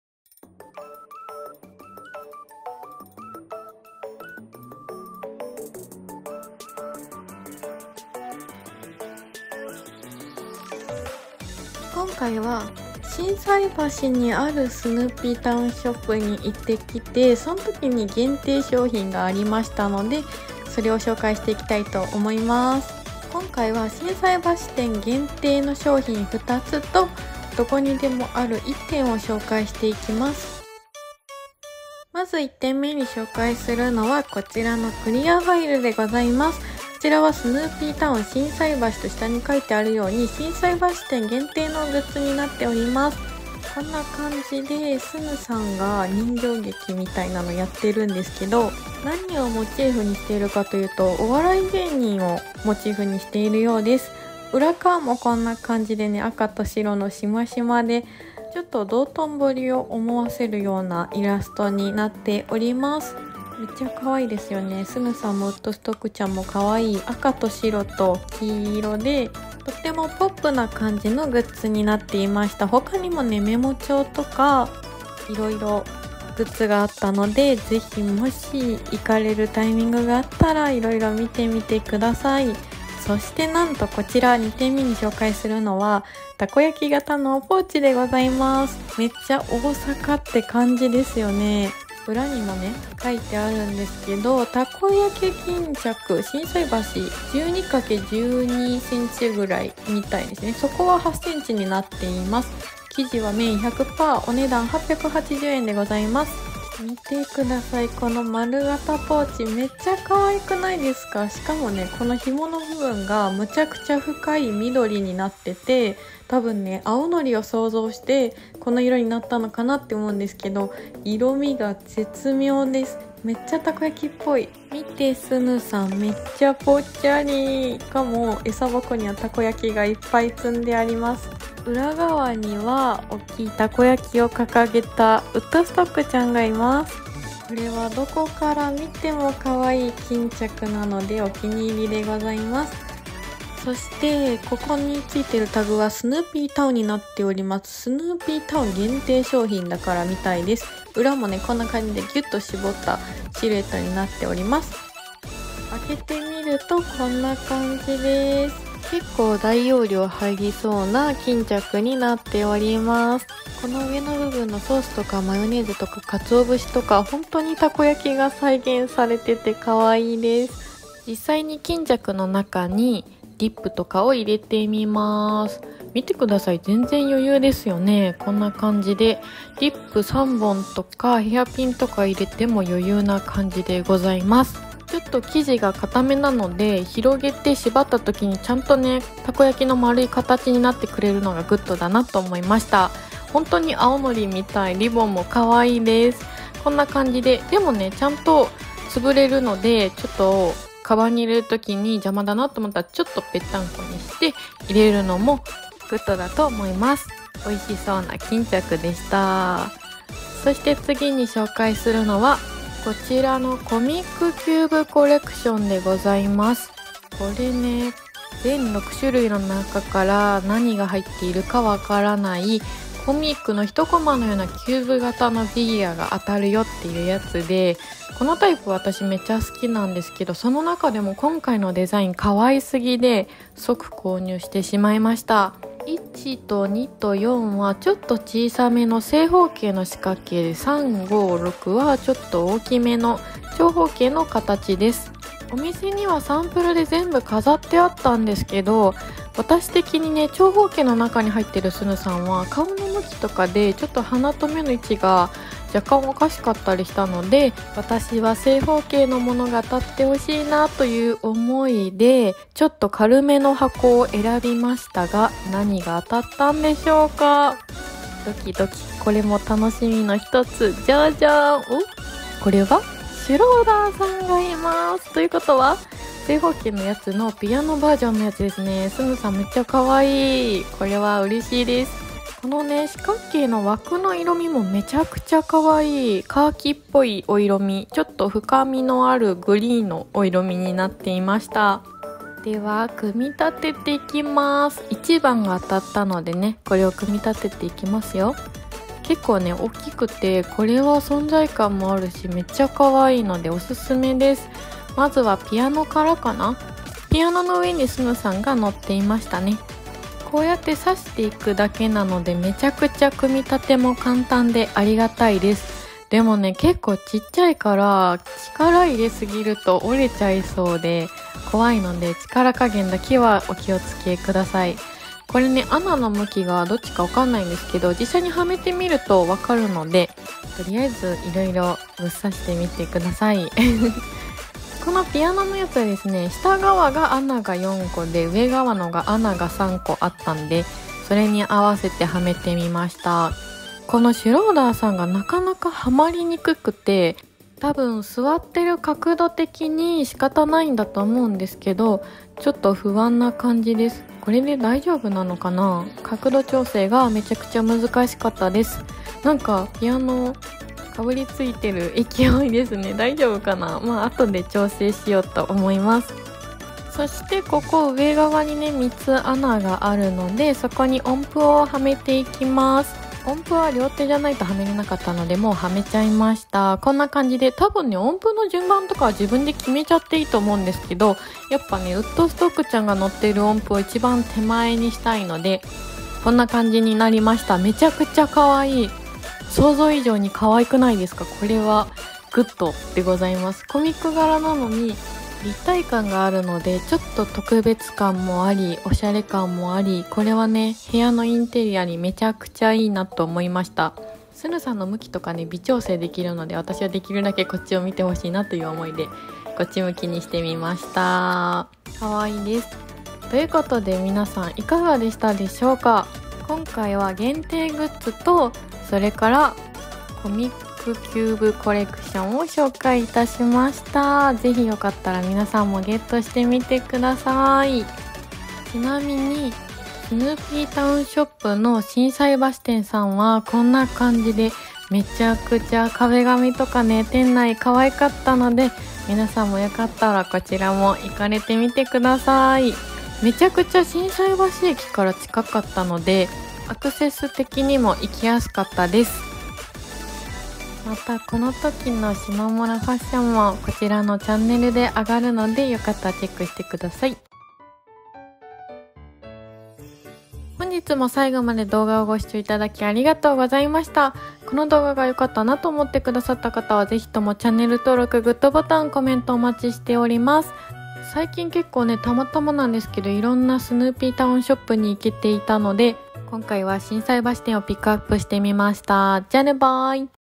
【スヌーピー】限定グッズ可愛すぎやろ🥺💓店内も最高✨【タウンショップ】
スヌーピータウンショップって、店舗によって雰囲気違いますよね☺️✨

tag→スヌーピー　限定 購入品紹介　　peanuts 　SNOOPY　スヌーピーグッズ　　コレクション インテリア　おうち時間　Collection Vlog 　ブイログ おしゃれ　大人　かわいい　日常　普段使い